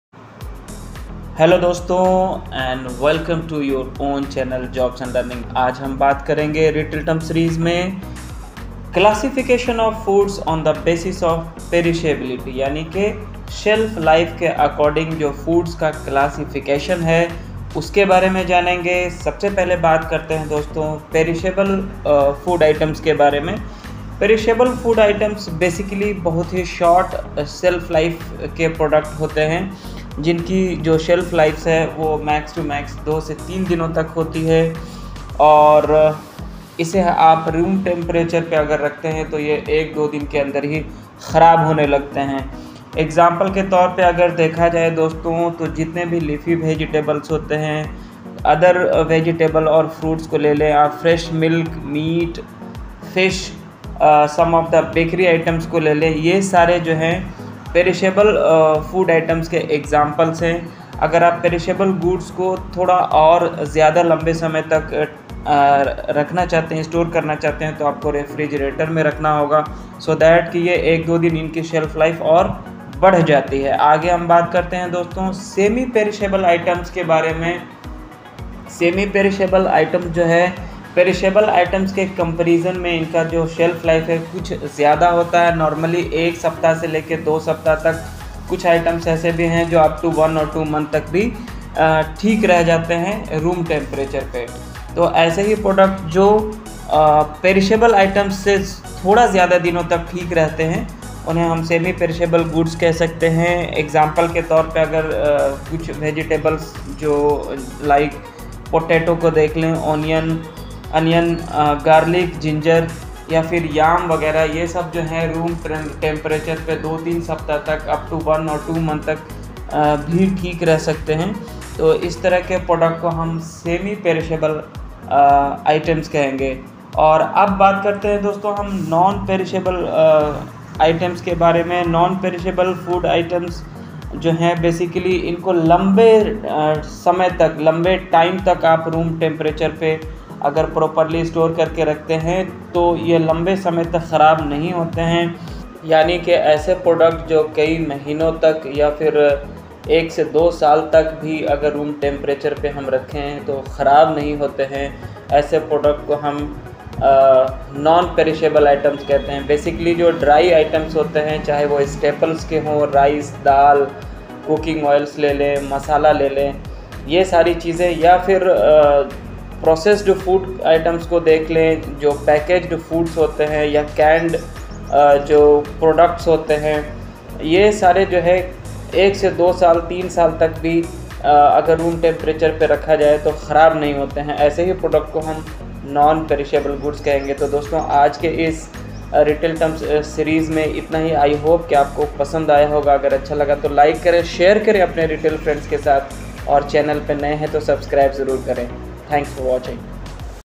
Perishable food's, Semi-Perishable food's, Non-Perishable food's, Retail terms
In this lesson we will know
about a term used in retail

Classification of food items on the basis of Perishability

Regards,
JOB'S&LEARNING